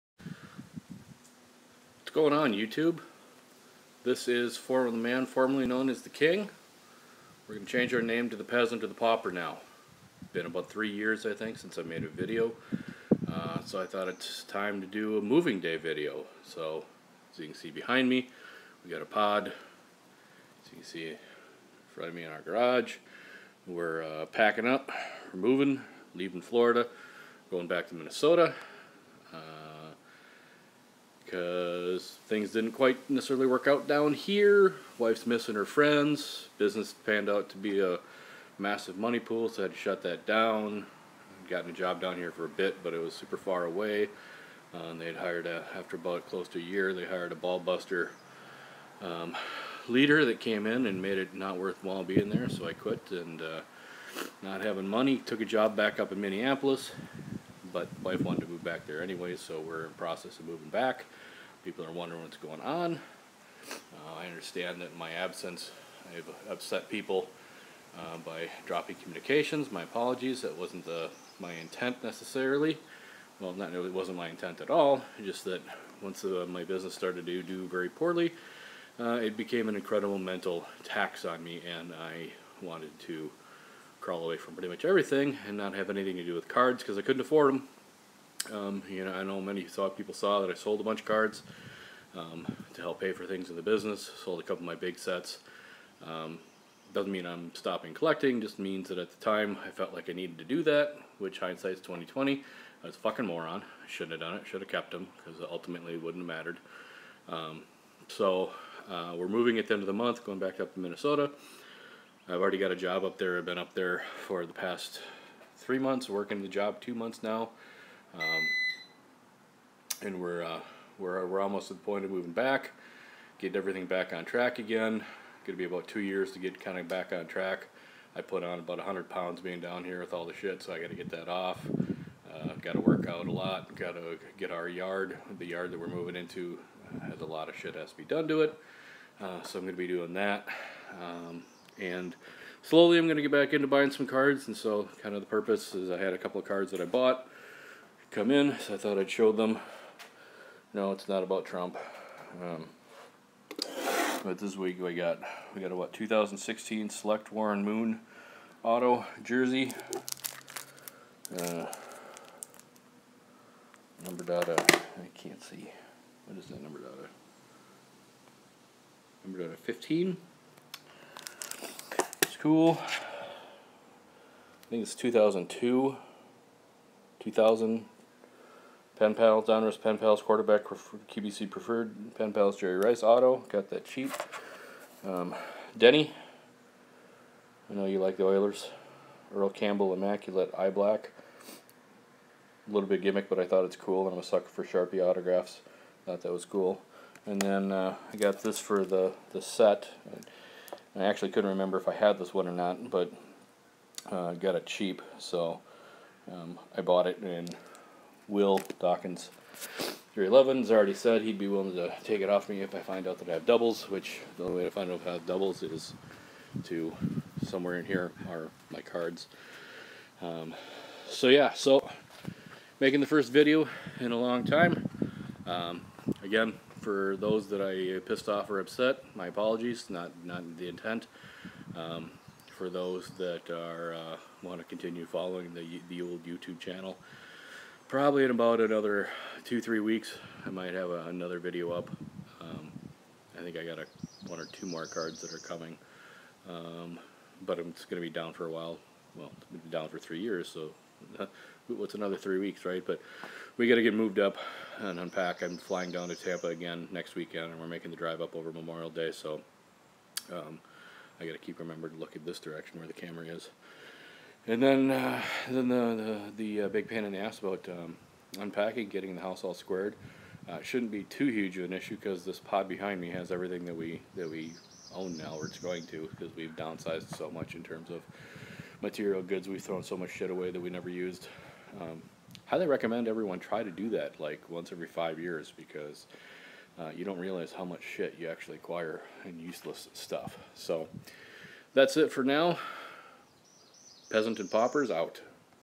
THE KING IS DEAD, INTRODUCING THE PAUPER MAILDAY
Been three years since I made a video, and I'd like to introduce the Peasant/Pauper